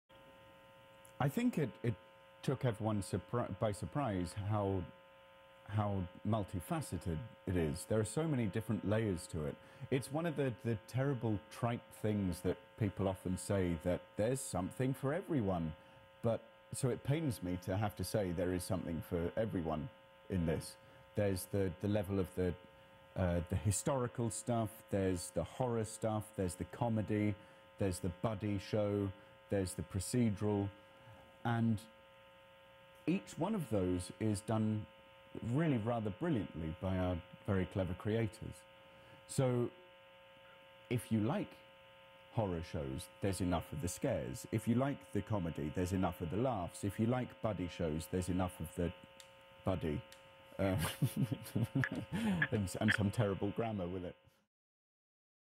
TOM MISON ON WHY 'SLEEPY HOLLOW' IS SO POPULAR
Star of the hit Fox series SLEEPY HOLLOW, TOM MISON offers an opinion on why the show is a fan favorite.

Season Two of SLEEPY HOLLOW premieres 9/22 on Fox-TV